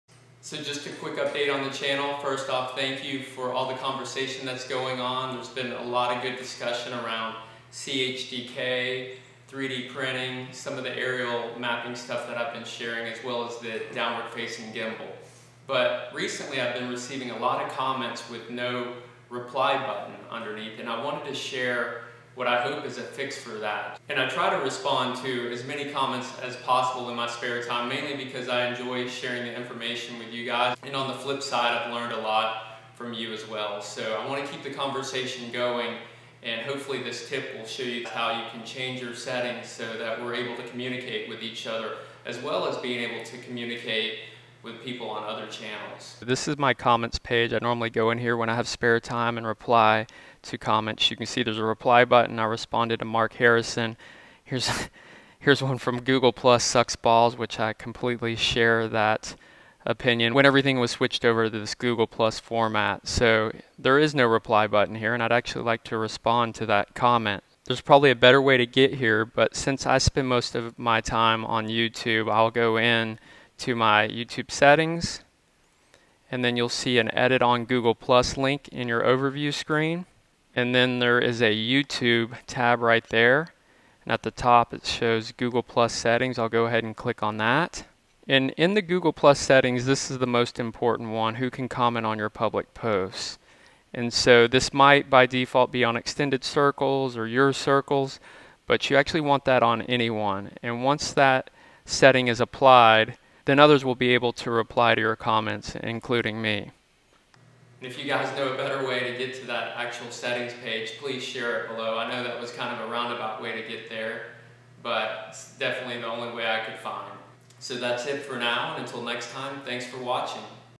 Can't Reply to Some YouTube Comments - Here's the Fix
I've getting more and more YouTube comments that I can't reply to. Hopefully this video shares how to overcome this. If you know a better way to get there please share it below and we'll upvote it. Thanks!